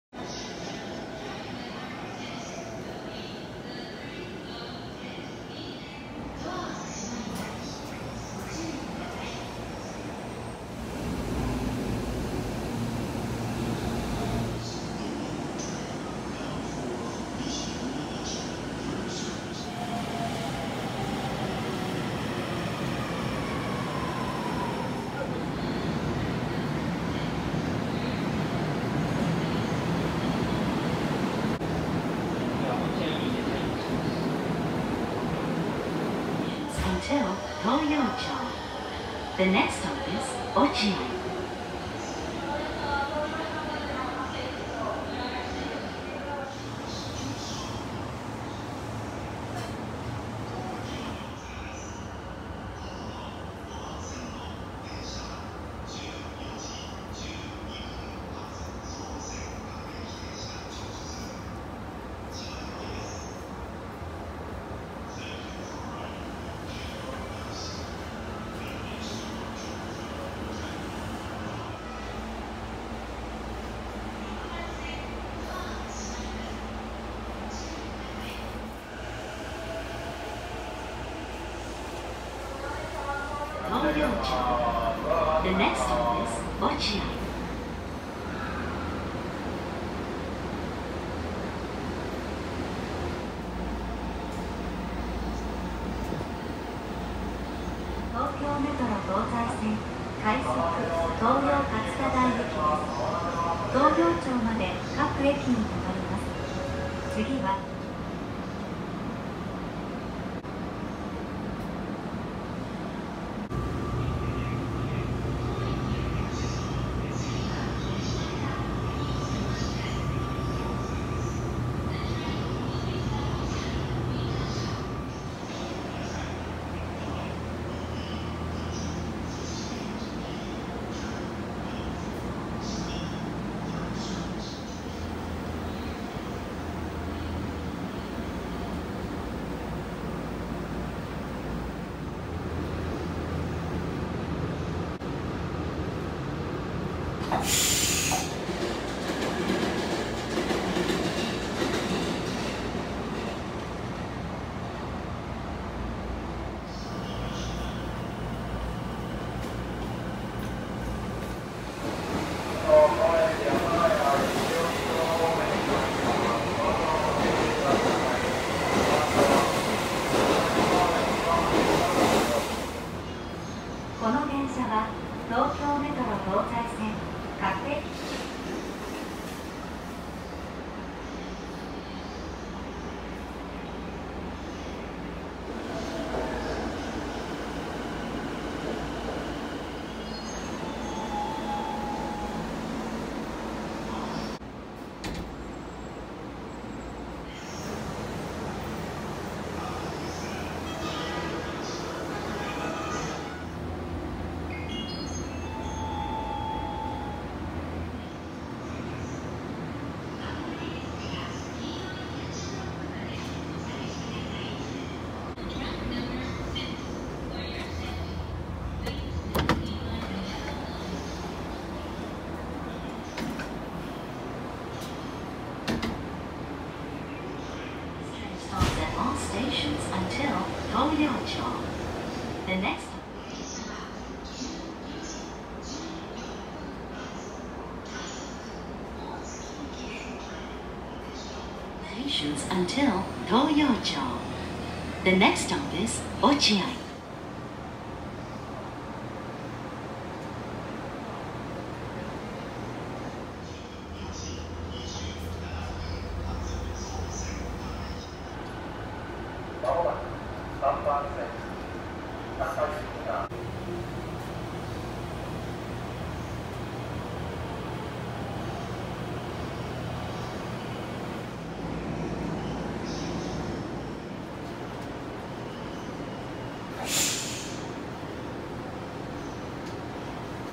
【車内を撮影】東京メトロ15000系を撮影しました
東京メトロ15000系を撮影しました。
ドアはワイドドアになっています。